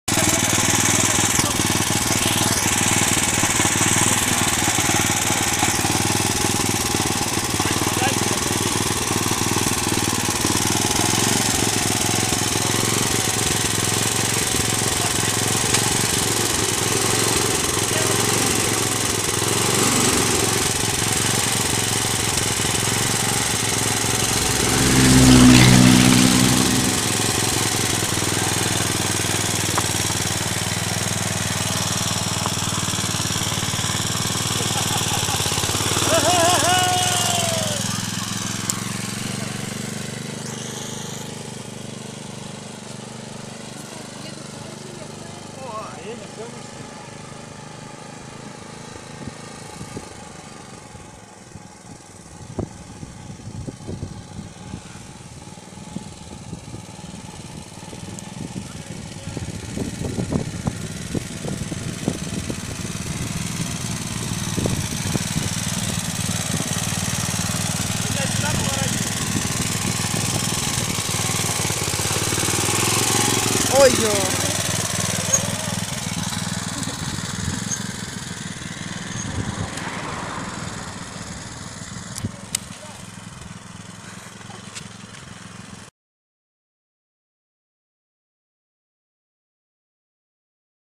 Деревенское такси от КТЗ. Минитрактор перевозит не только грузы.
Деревенское такси от КтЗ.
Мотоблок + передний адаптер = минитрактор.
А с таким транспортом можно не только грузы перевозить, но и пассажиров. Передний адаптер для мотоблока ктз показывает на что еще он способен в случае чего.

Следите за каналом! Благодарю за просмотр!